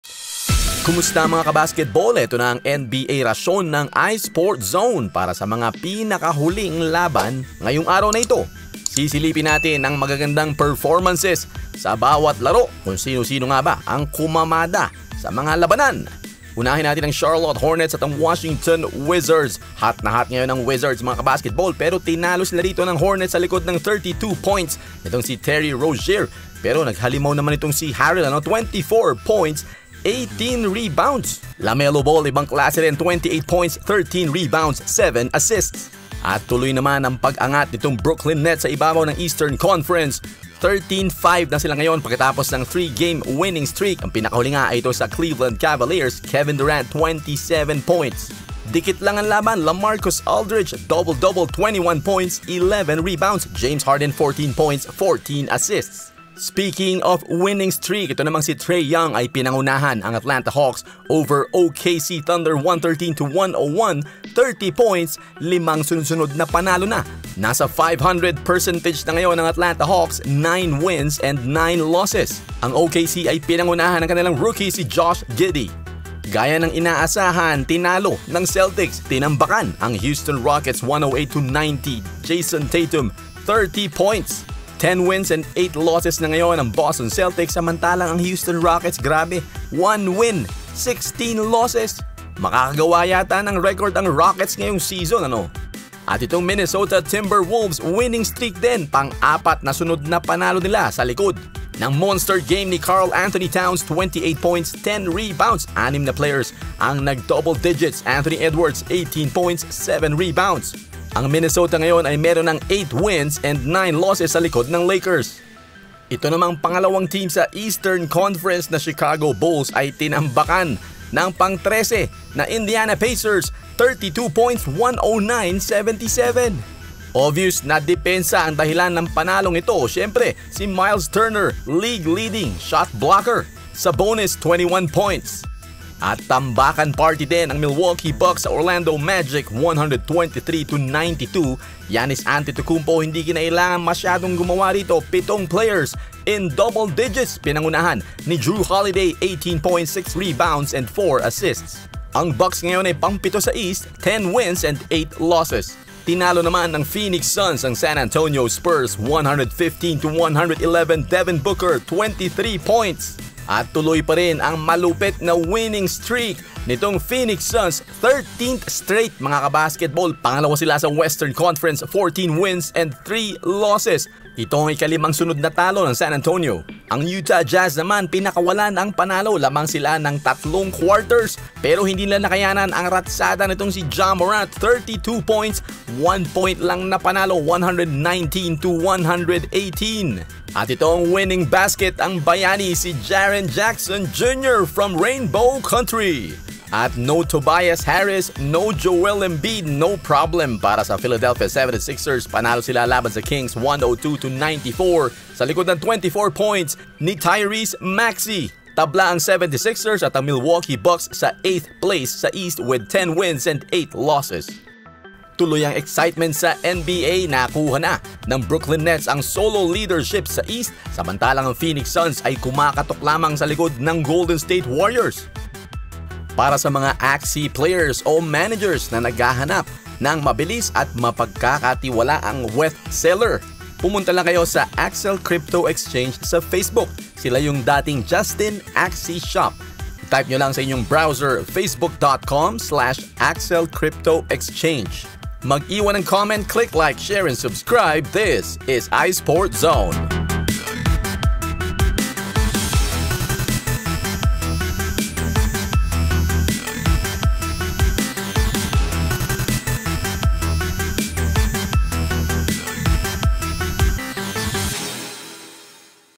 Durant, Harden at Nets Balik sa IBABAW ng East. Phoenix Suns GRABE sa 13 Straight Win.
Nakuha na nila Kevin Durant, James Harden at Brooklyn Nets ang solo leadership sa East. Ang Phoenix Suns naman 13th sunod na panalo.

Accel Crypto Exchange
Nagbebenta ng WETH at AXS.

Matutong mag vlog o YouTube! Kumita ng pera. Visit my graphy for live seminars and tutorials